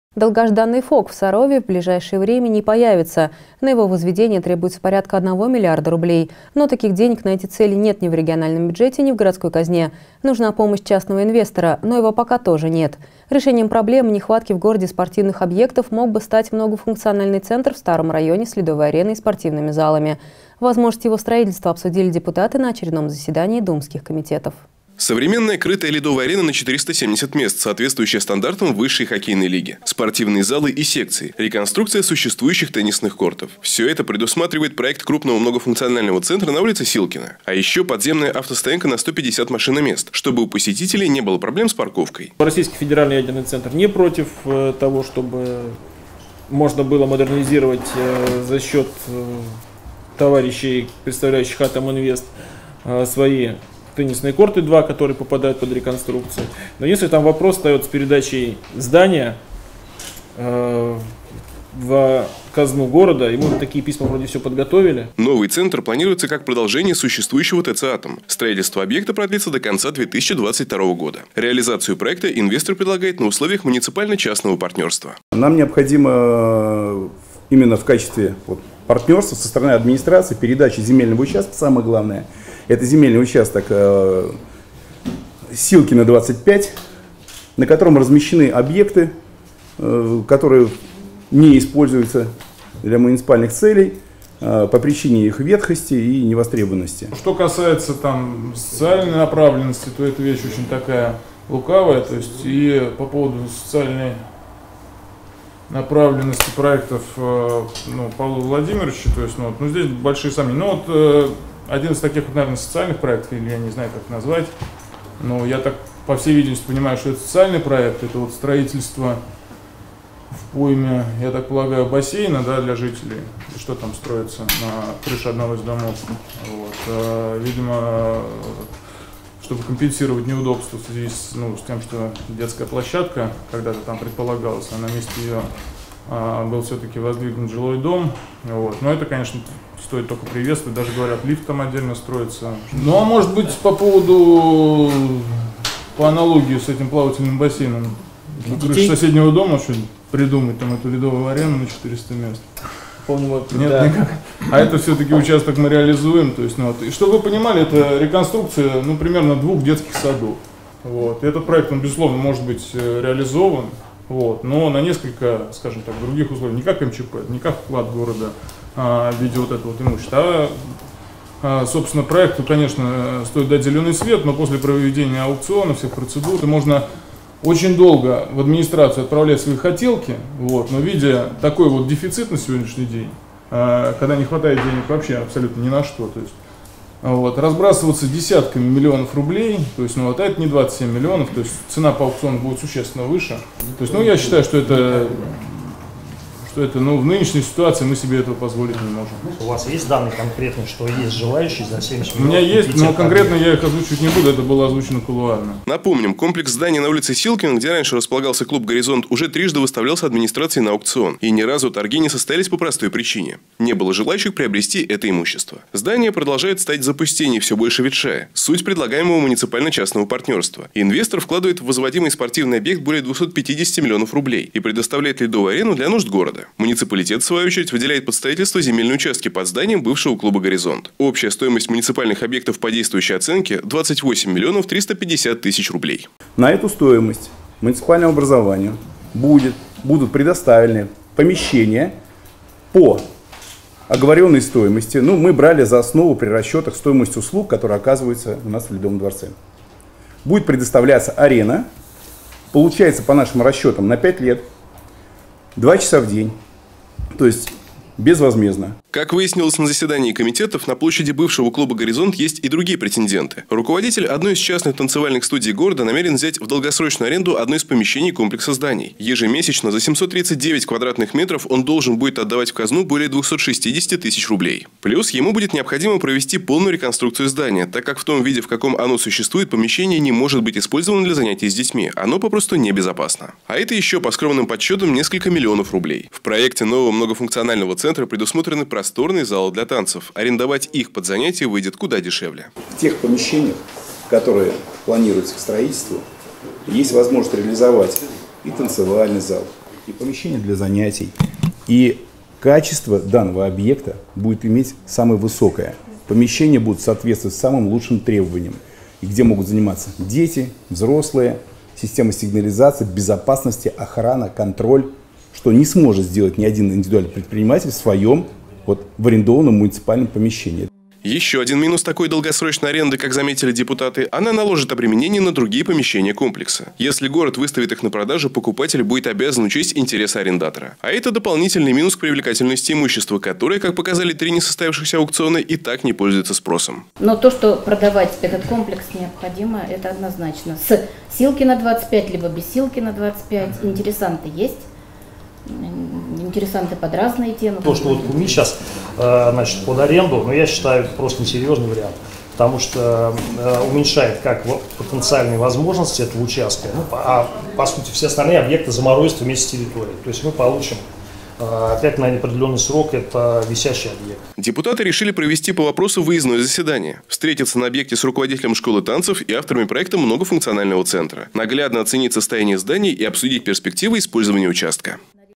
Многофункциональный центр с ледовой ареной может появиться в Сарове
Долгожданный ФОК в Сарове в ближайшее время не появится. На его возведение требуется порядка 1 миллиарда рублей. Но таких денег на эти цели нет ни в региональном бюджете, ни в городской казне. Нужна помощь частного инвестора. Но его пока тоже нет. Решением проблемы нехватки в городе спортивных объектов мог бы стать многофункциональный центр в старом районе с ледовой ареной и спортивными залами. Возможность его строительства обсудили депутаты на очередном заседании думских комитетов.